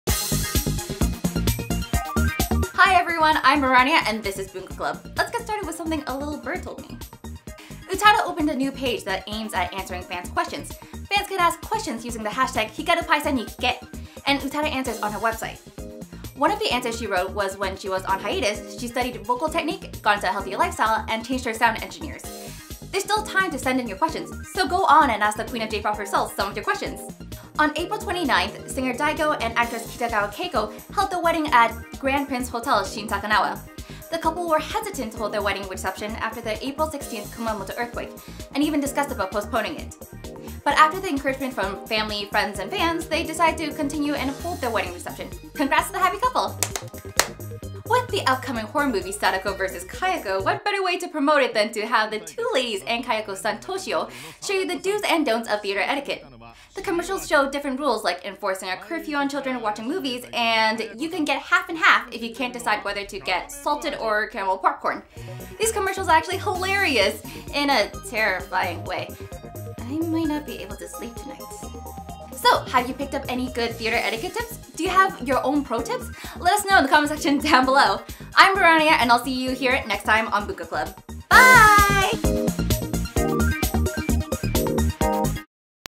BUNKA CLUB: Utada Hikaru's PAISEN, DAIGO and Kitagawa Keiko, plus Theatre Etiquette
BUNKA CLUB! Mirania is back for another news!

Utada Hikaru's been answer fans question through twitter on her website.

DAIGO and Kitagawa Keiko (known as Sailor Mars in the Sailor Moon Live Action back in mid 2000's) gets married.

Kayako and Toshio returns from the TV screen not to scare you, but this time, to tell you theatre etiquette prior to your highly anticipated movie you're about to watch. Quite hilarious we find.